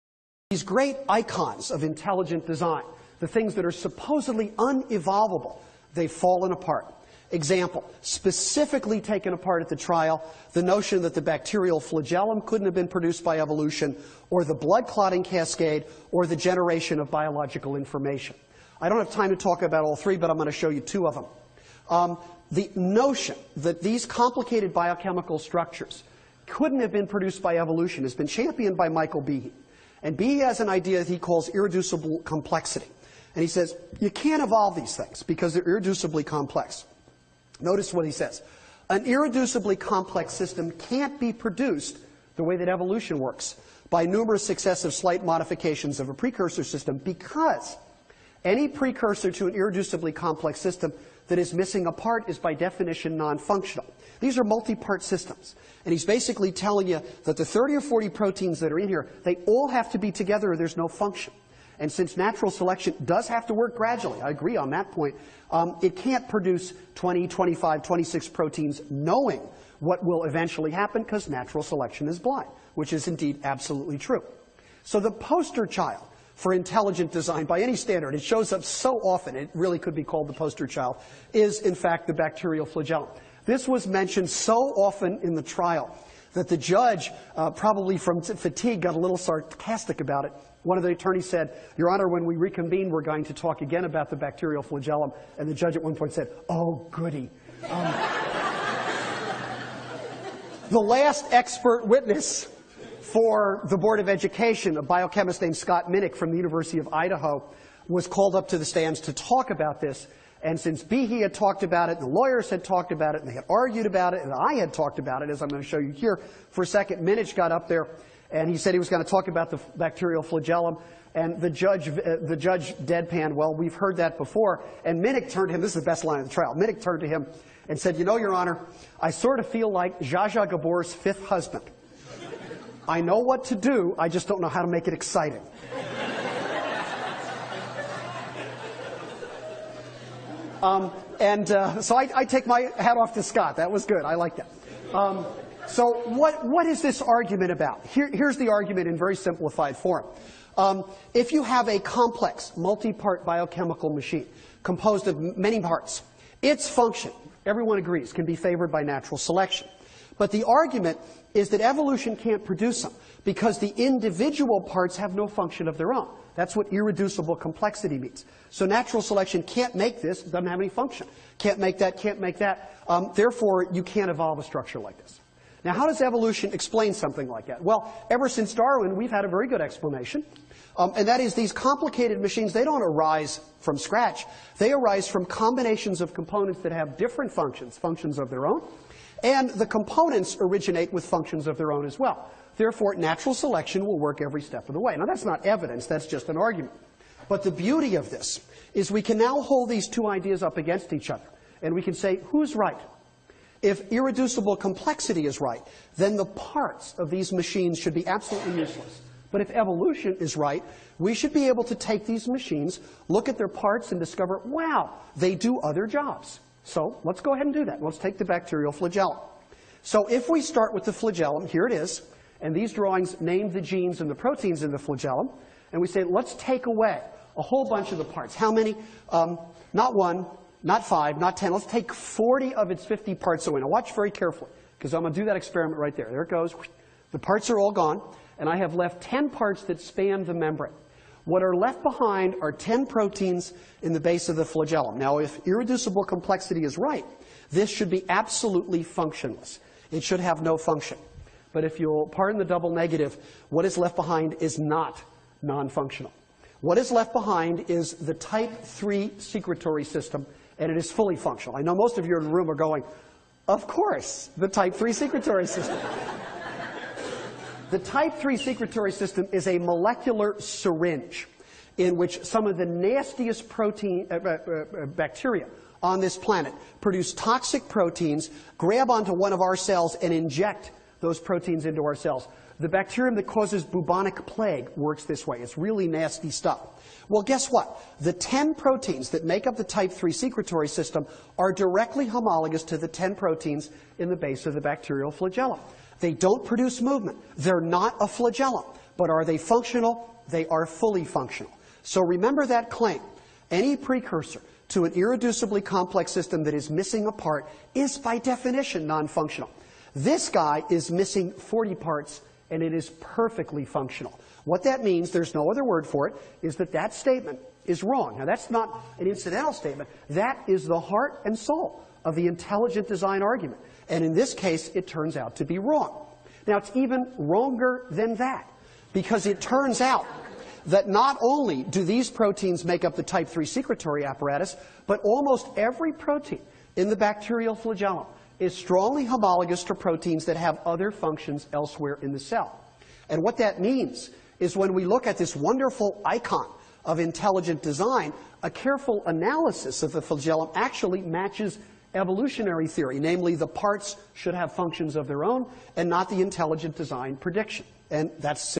تساقط حجج التصميم الذكي - السوط البكتيري
هذه الأيقونات العظيمة للـ ‫#‏التصميم_الذكي‬
السوط البكتيري ما بين التطور والتصميم الذكي ..هل يمثل حقا عقبة أمام التطور ؟
الأشياء التي من المفروض أنه لا يمكن لها أن تتطور، إنها تتساقط

من تقديم العالم : كينيث ميلر